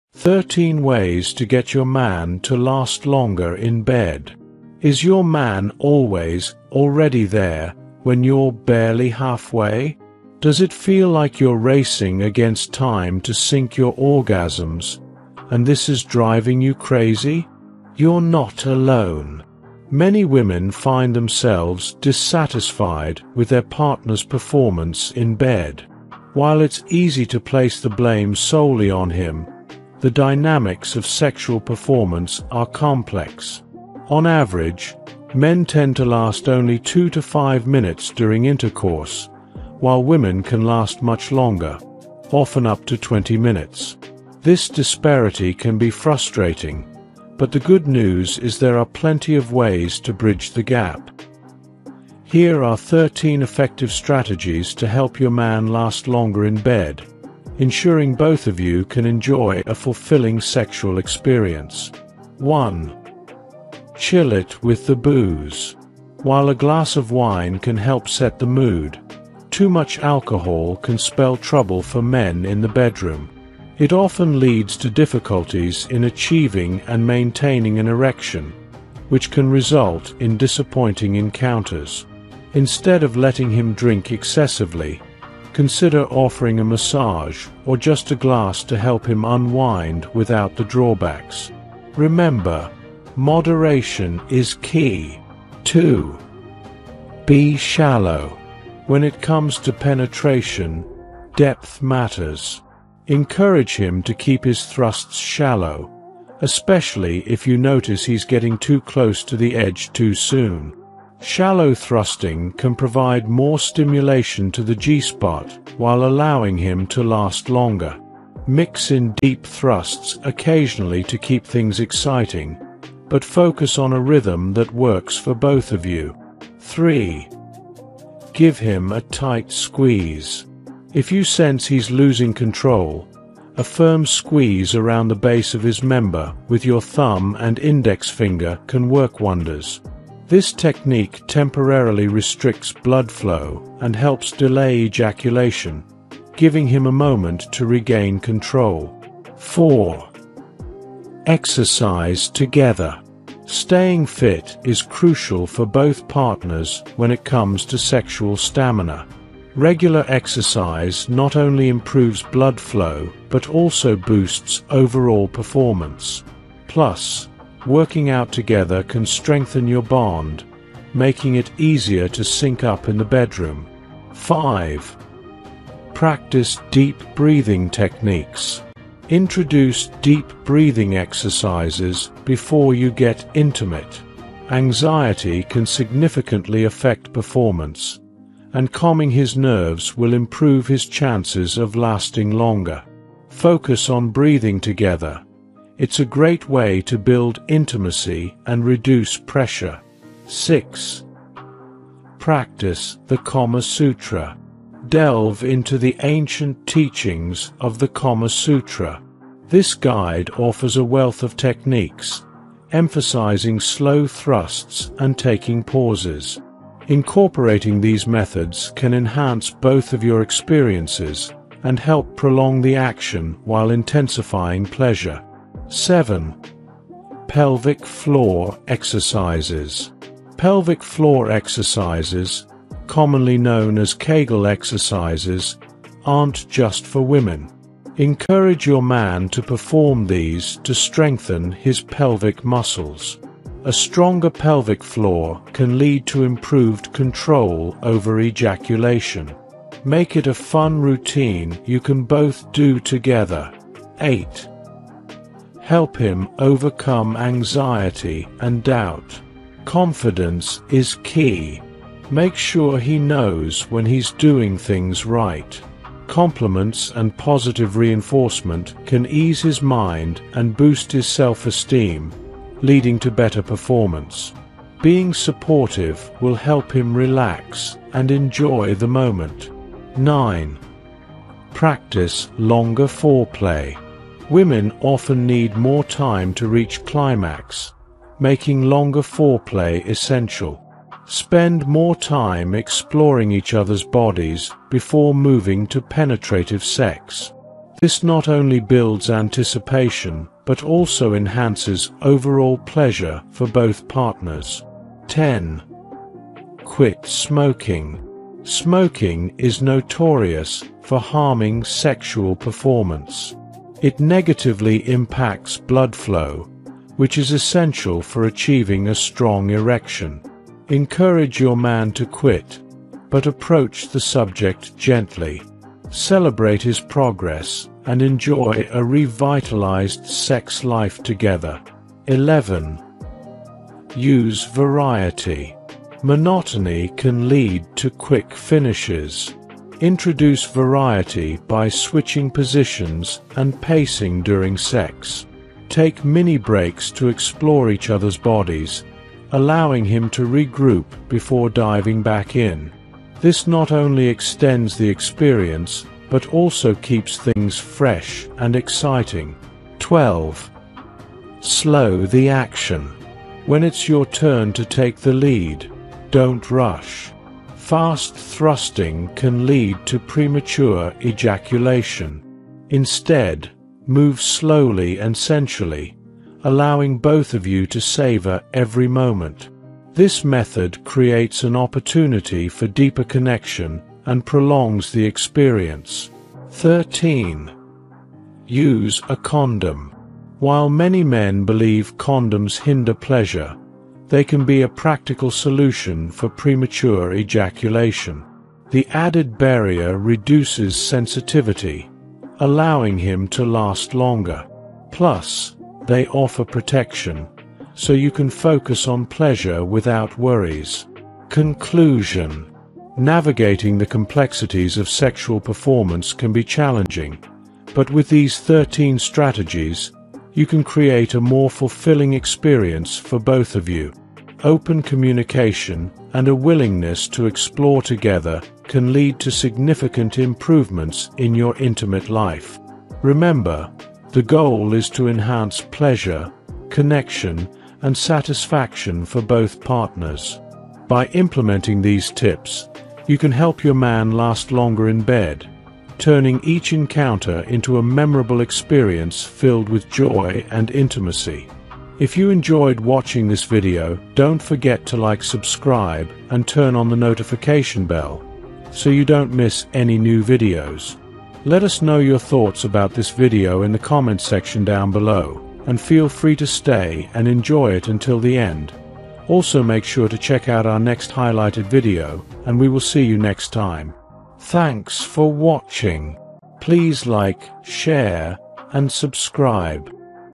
13 Proven Ways to Help Your Man Last Longer in Bed and Enhance Your Pleasure | Psychology Facts
13 Proven Ways to Help Your Man Last Longer in Bed and Enhance Your Pleasure | Psychology Facts
In today's video, we're diving deep into an important topic that many couples struggle with—how to help your man last longer in bed. If you've ever found yourself feeling frustrated by a quick encounter, you're not alone. Many women experience this, but the good news is that there are effective strategies to enhance sexual stamina and intimacy.

Did you know that the average man lasts only two to five minutes during sex? While this can be disappointing, understanding the reasons behind this can help you both explore solutions together. In this video, we’ll share 13 practical tips that can make a significant difference in your bedroom experience.

1. Chill It with the Booze: While a little alcohol can help you feel relaxed, too much can hinder performance. Discover how moderation can enhance intimacy without compromising his stamina.

2. Be Shallow: Learn the benefits of shallow thrusts. This technique not only prolongs the experience but also enhances pleasure for both partners.

3. Give Him a Tight Squeeze: This simple trick can help him control his climax. We'll show you when and how to use this technique effectively.

4. Exercise Together: Regular physical activity improves stamina and blood flow, both essential for better sexual performance. Find out which exercises you can enjoy as a couple!

5. Practice Deep Breathing: Anxiety can negatively impact performance. We’ll guide you through breathing techniques that can help both of you stay relaxed and connected.

6. Explore the Kama Sutra: This ancient text is full of innovative techniques. We’ll discuss specific methods that can help prolong your intimacy sessions.

7. Try Pelvic Floor Exercises: Known as Kegel exercises, these can enhance both of your experiences. We'll explain how men can benefit from these too!

8. Overcome Anxiety Together: Boost your man's confidence by recognizing his efforts in bed. We'll share tips on how to communicate positively and effectively.

9. Extend Foreplay: Understand the importance of longer foreplay to help sync your orgasms. Find out how this can lead to a more satisfying experience for both partners.

10. Quit Smoking: Smoking can adversely affect sexual health. We’ll discuss gentle ways to support your partner in quitting for better performance.

11. Use Variety: Explore different positions and techniques to keep things exciting and prolong your encounters. We'll provide examples and suggestions.

12. Slow Down the Action: Learn why taking it slow can significantly enhance pleasure for both of you. This is about savoring every moment together.

13. Use a Condom: Discover how condoms can actually help with premature ejaculation, leading to longer-lasting encounters.

Whether you're looking to improve your intimacy or enhance your overall relationship, these tips are designed to make your experiences in the bedroom more fulfilling. Remember, communication is key, and trying these techniques together can strengthen your bond.

Key Takeaway
Together, you can create a more satisfying sexual experience that strengthens your connection. Join us on this journey towards better intimacy, and let's explore these 13 ways to enhance your pleasure in bed!